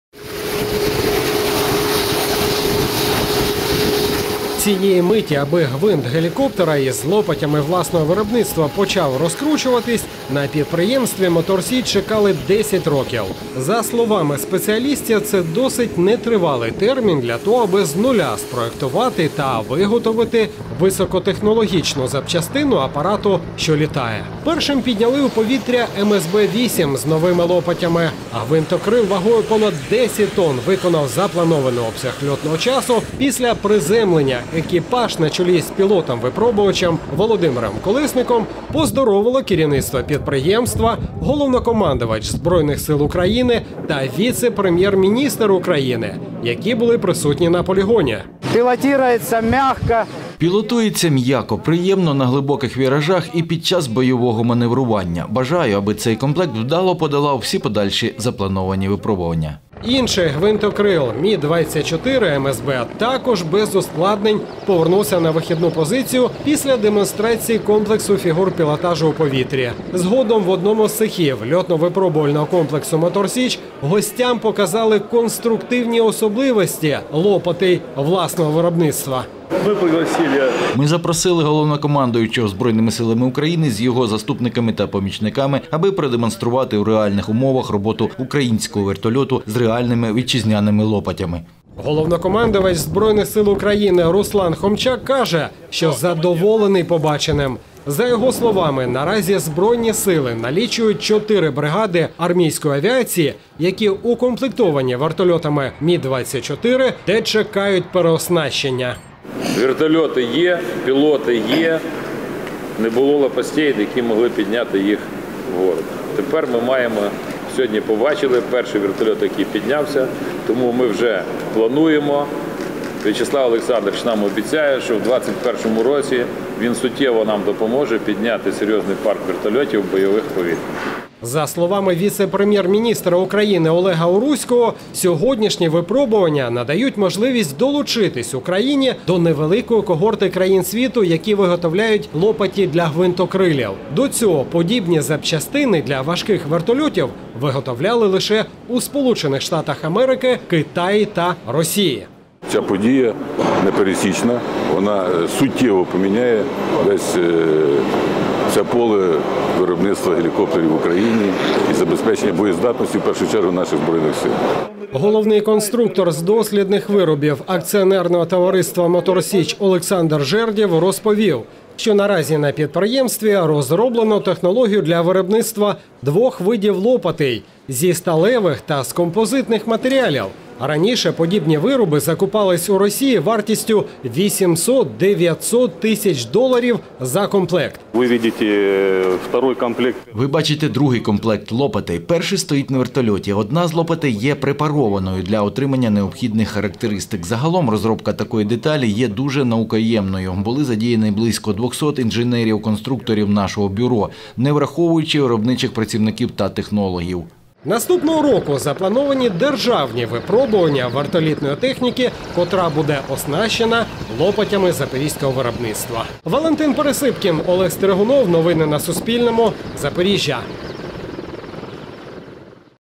Нові лопаті "Мотор Січі" | Новини | 24.12.2020
У Запоріжжі випробувували нові вертолітні лопаті, котрі незабаром планує серійно випускати місцеве підприємство «Мотор Січ». На льотно-випробувальному комплексі «Мотор Січ» у повітря піднялося два важких вертольоти, оснащених лопатями власного виробництва.